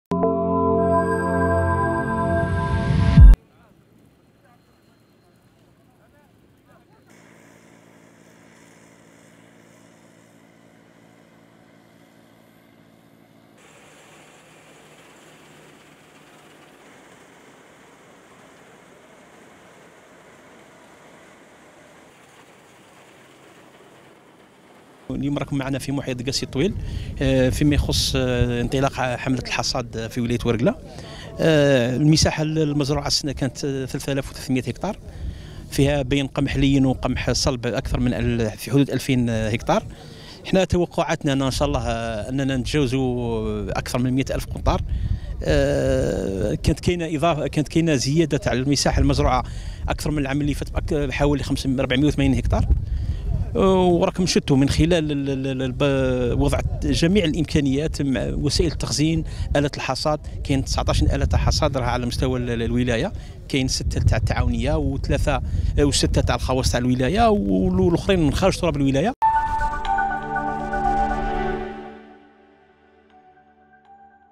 حملة الحصاد والدرس: أكثر من 100 ألف قنطار من الحبوب منتظرة بولاية ورقلة
ورقلة (وأج)- ينتظر تحقيق محاصيل بأكثر من 100 ألف قنطار من مختلف أصناف الحبوب بولاية ورقلة في إطار حملة الحصاد والدرس التي أطلقت لهذا الموسم.